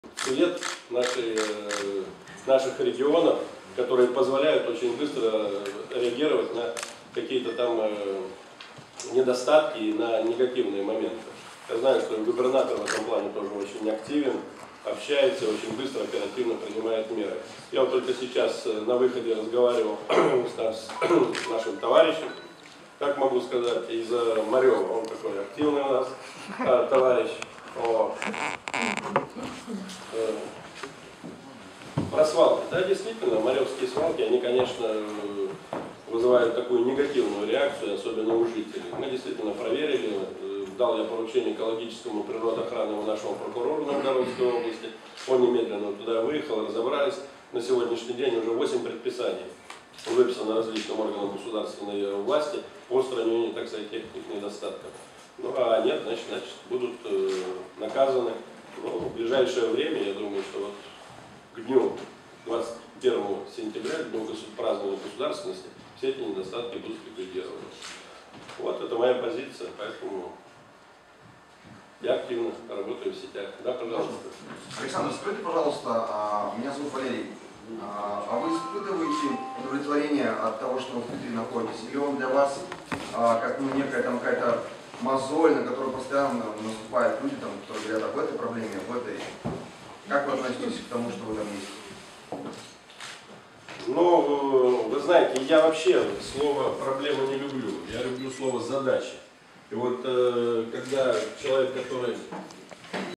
Сенатор Александр Коровников на встрече с блогерами
Встреча сенатора Александра Коровникова с питерскими блогерами в Великом Новгороде. Видео - Андрей Хаустов, newreportage.ru